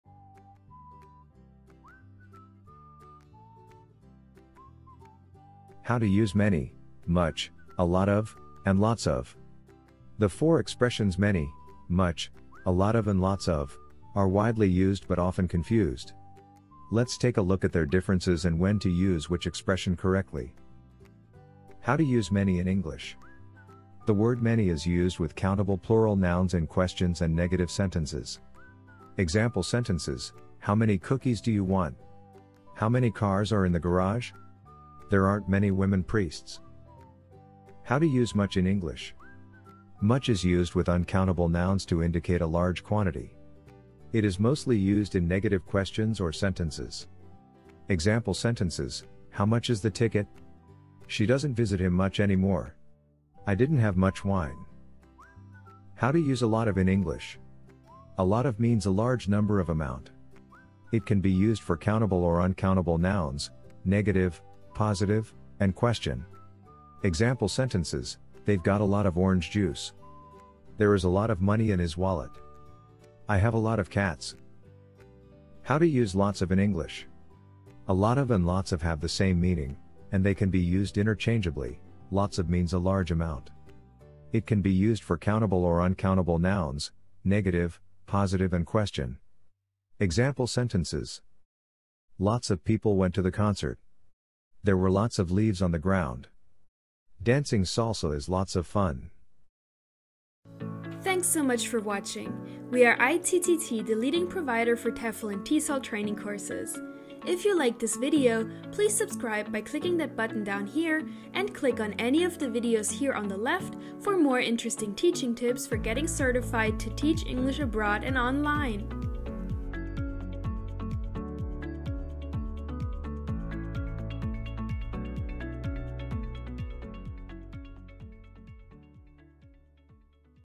English Grammar: How to Use Many, Much, A lot of, and Lots of | ITTT | TEFL Blog
English Grammar: Much, Many, etc. The four expressions "many", "much", "a lot of" and "lots of" are widely used but often confused. Let's take a look at their differences and when to use which expression correctly.

Are you ready to live and teach abroad?

What is TEFL Certification? Where is it valid?